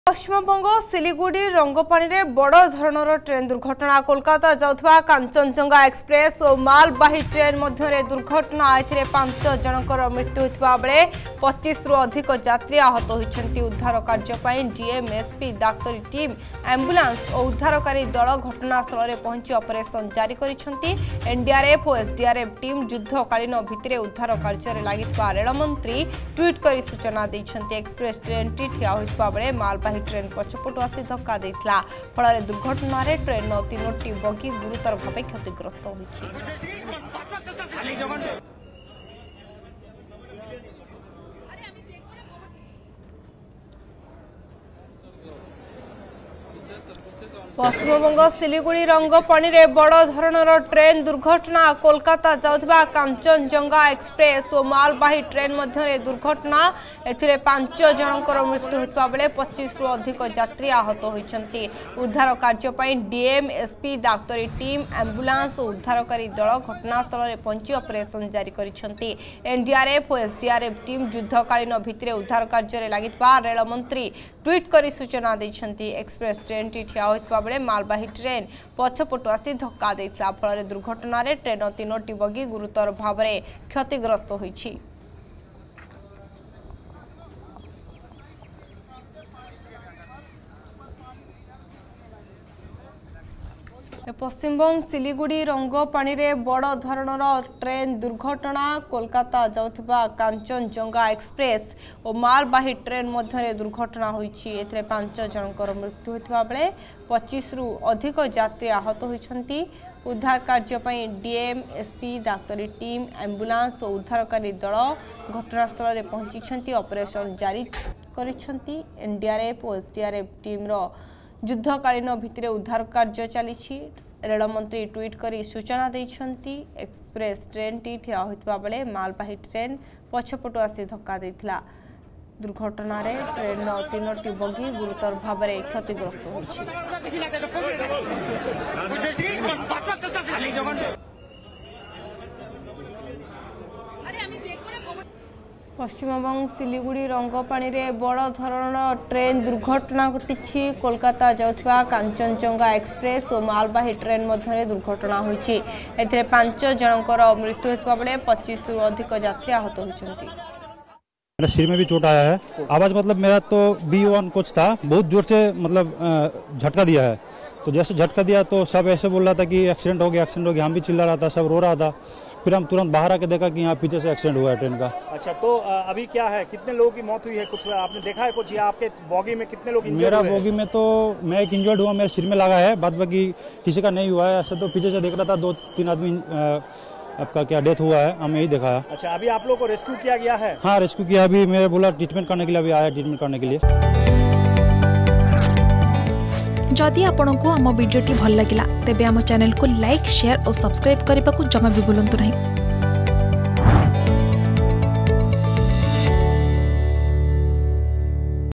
ଦାର୍ଜିଲିଂରେ ରେଳ ଦୁର୍ଘଟଣା || West Bengal Train Accident
ପଶ୍ଚିମବଙ୍ଗ ସିଲିଗୁଡ଼ି ରଙ୍ଗପାଣିରେ ବଡ଼ ଧରଣର ଟ୍ରେନ ଦୁର୍ଘଟଣା । କୋଲକାତା ଯାଉଥିବା କାଞ୍ଚନଜଙ୍ଗା ଏକ୍ସପ୍ରେସ ଓ ମାଲବାହୀ ଟ୍ରେନ ମଧ୍ୟରେ ଦୁର୍ଘଟଣା। ଏଥିରେ ୫ ଜଣଙ୍କର ମୃତ୍ୟୁ ହୋଇଥିବା ବେଳେ ୨୫ରୁ ଅଧିକ ଯାତ୍ରୀ ଆହତ ହୋଇଛନ୍ତି ।